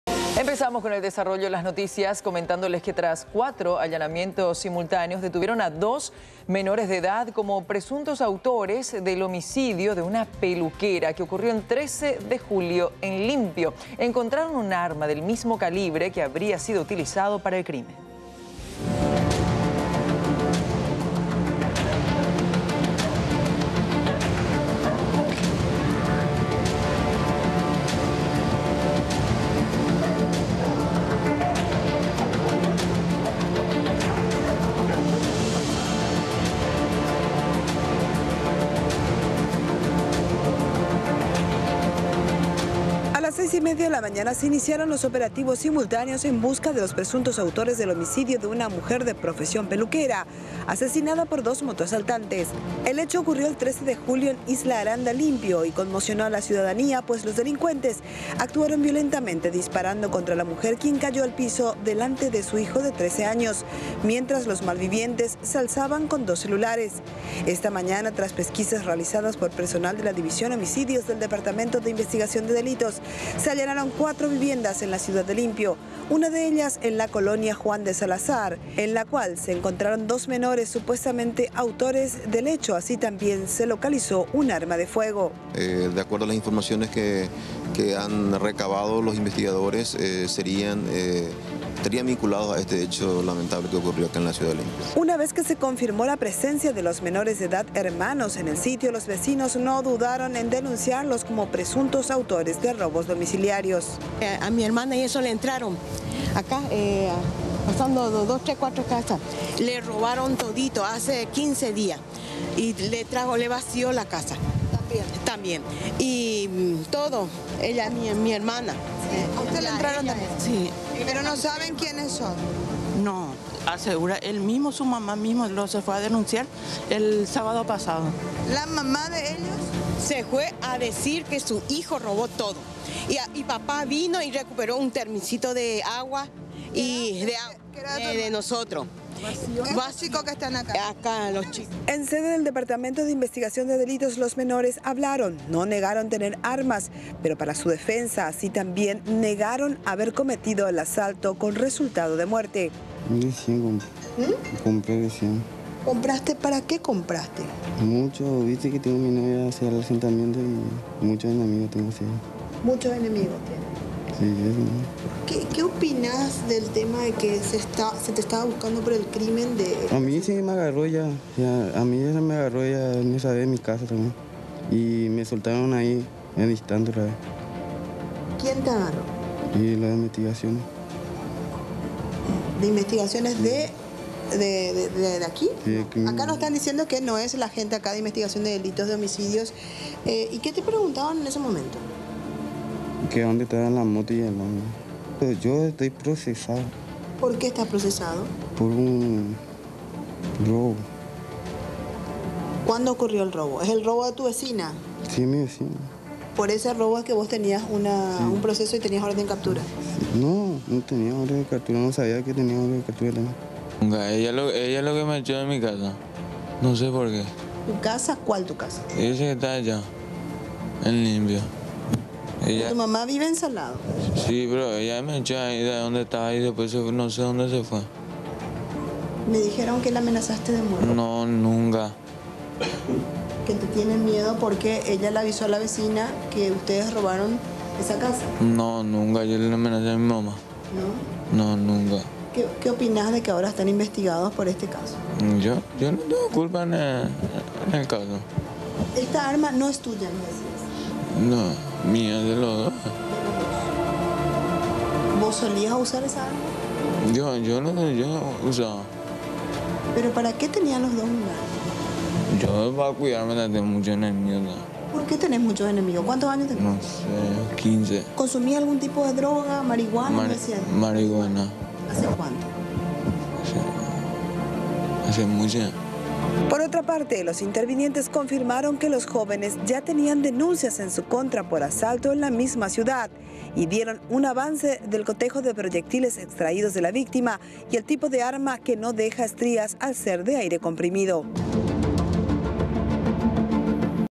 Dos menores de edad detenidos por caso de peluquera asesinada
Cuatro allanamientos simultáneos dieron con los sospechosos. El arma fue un determinante para la detención. La muerte de la peluquera ocurrió el 13 de julio. La mujer murió delante de su hijo de 13 años.